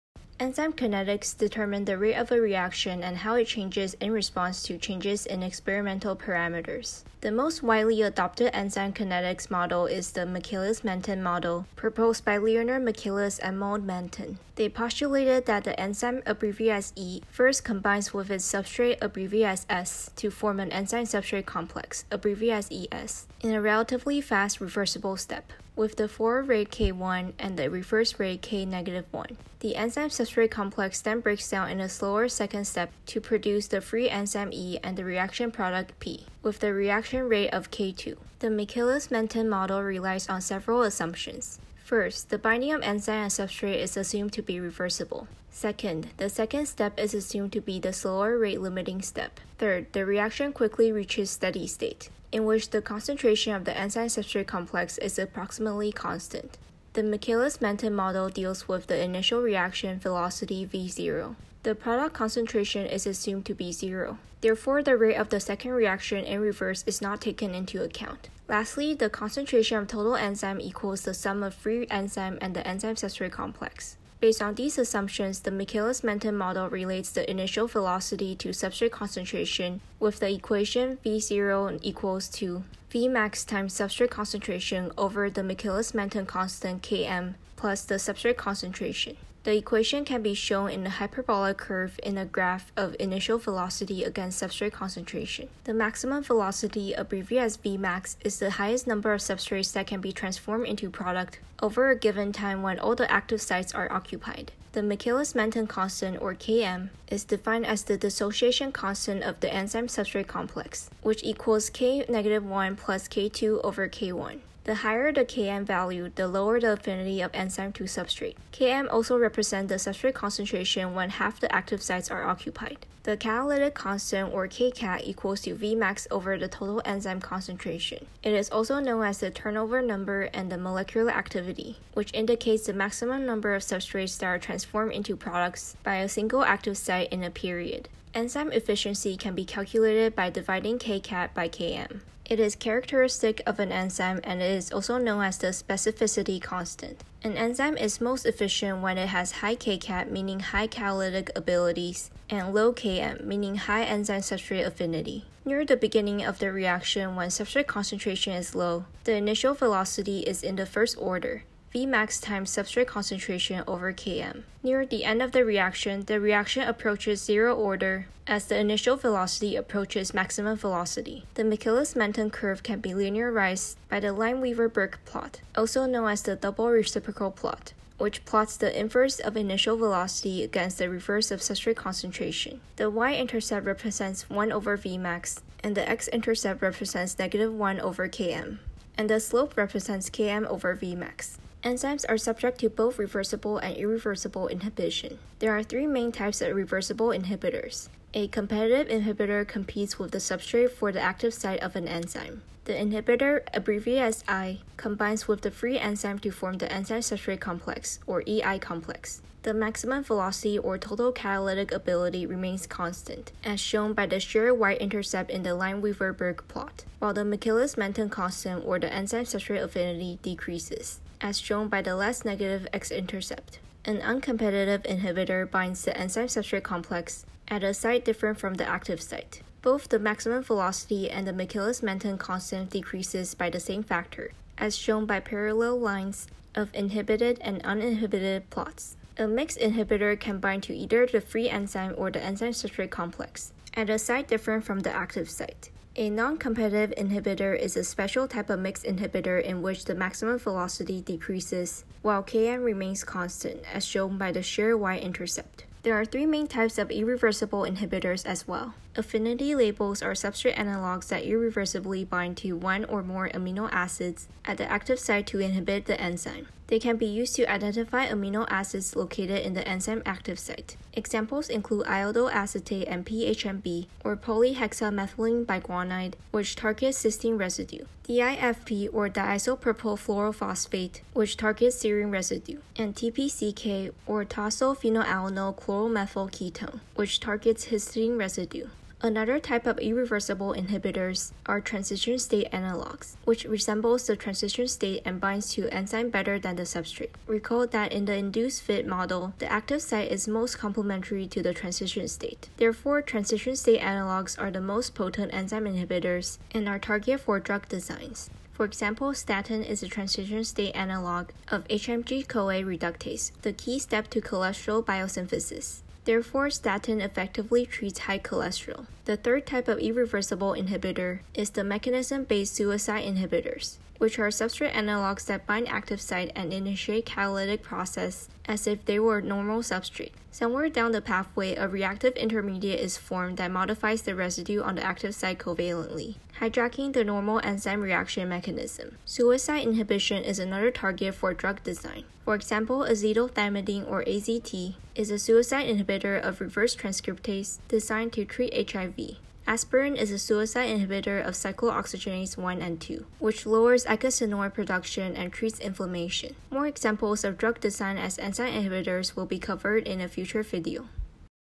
Enzyme Kinetics | Enzymes 03 | Biochemistry | PP Notes | Lehninger 6E Ch. 6.3
A summary review video about enzyme kinetics.

Timestamps:
0:00 Michaelis-Menten Model
3:11 Reversible Inhibition (Competitive, Uncompetitive, Noncompetitive)
4:15 Irreversible Inhibition (Substrate Analogs, Transition-State Analogs, Suicide Inhibitors)